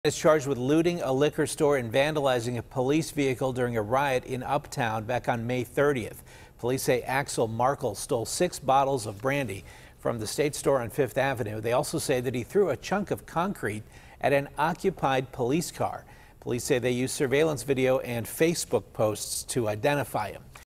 Police Arrest Looting Suspect
A man is being charged with looting a liquor store during a riot in Uptown on May 30.